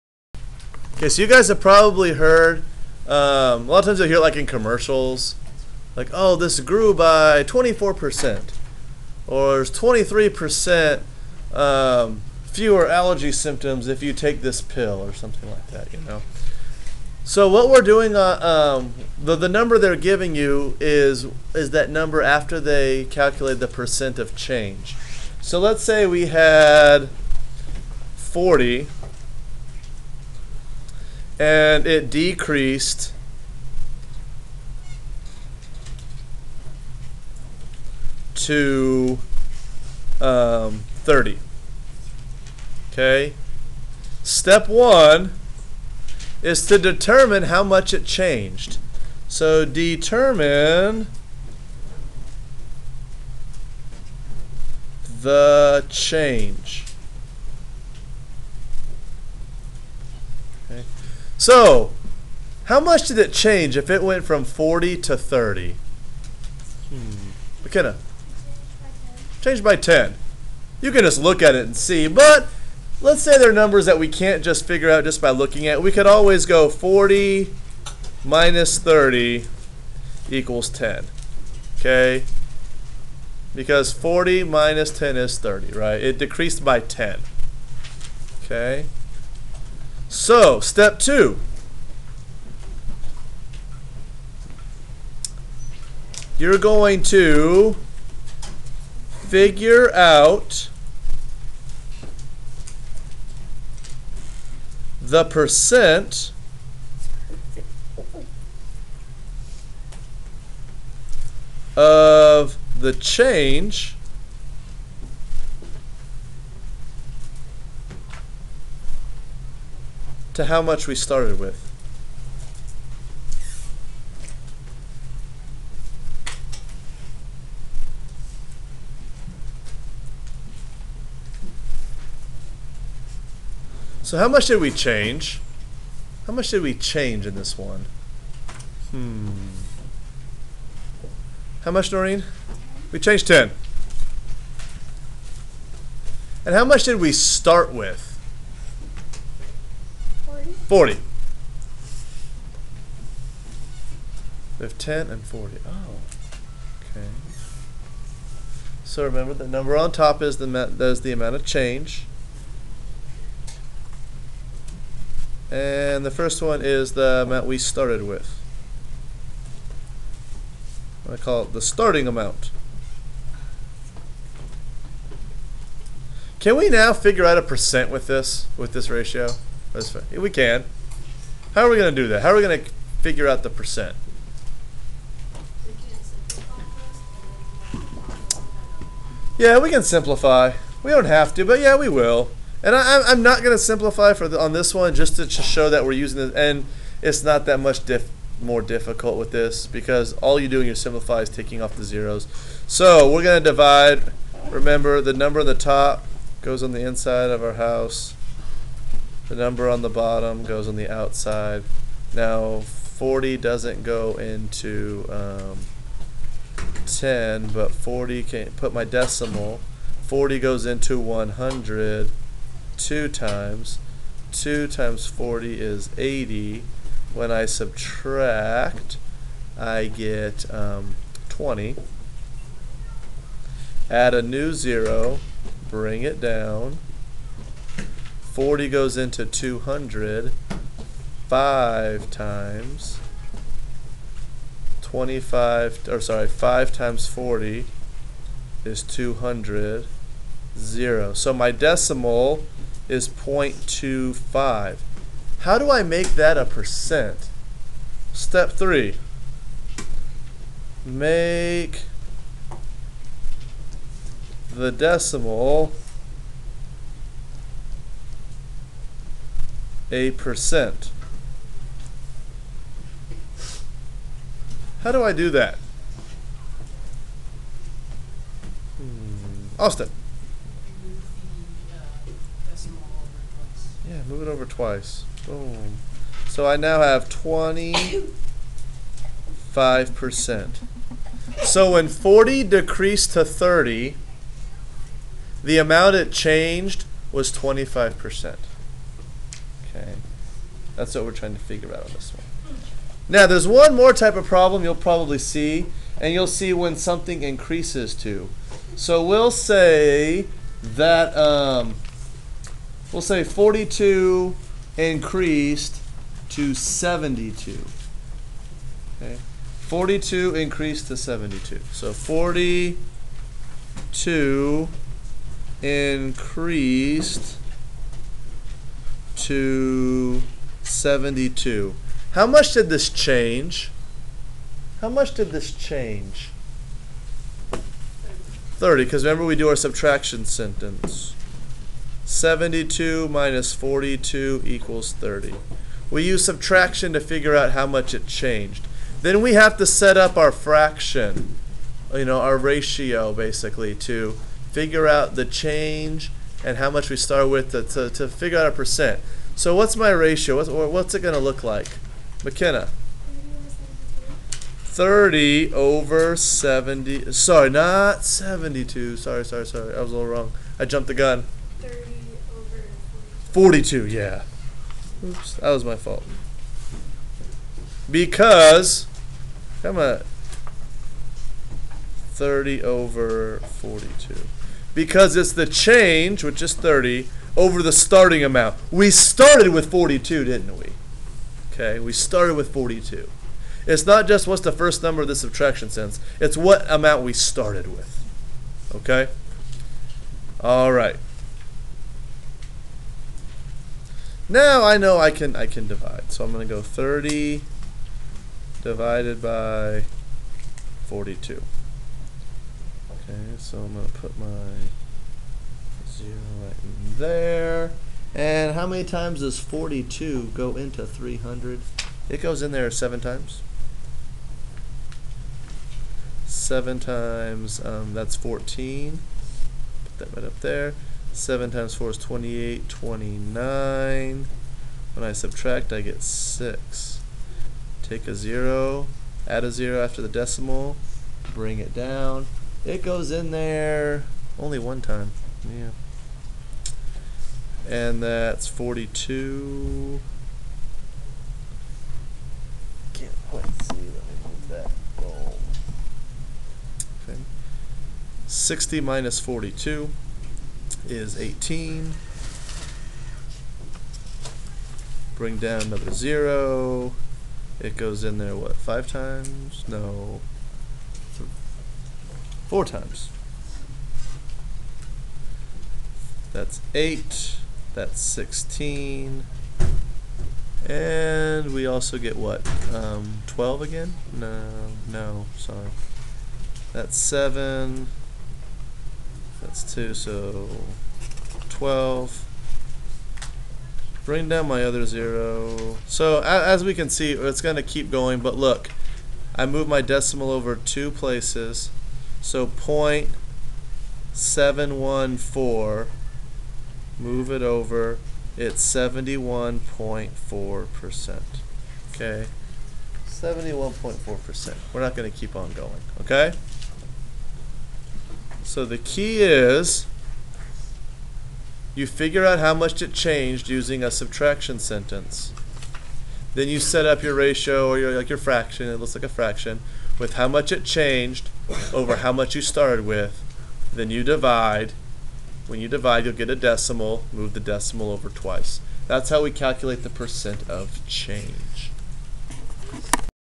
Finding Percent Changes
This tutorial helps figure out how to find to find the percent change in amounts.